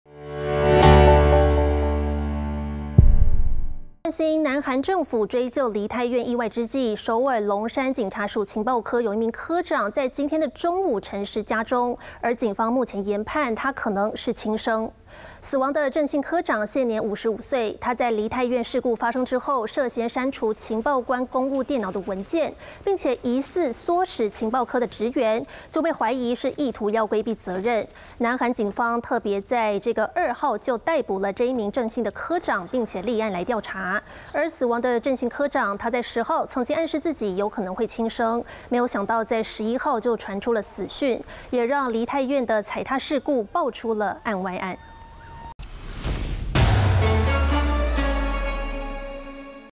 涉刪除梨泰院踩踏報告 龍山警署科長疑輕生亡｜華視新聞 20221111
南韓政府追究梨泰院意外之際，首爾龍山警察署情報科一名科長，11日午間陳屍家中，警方研判可能是輕生。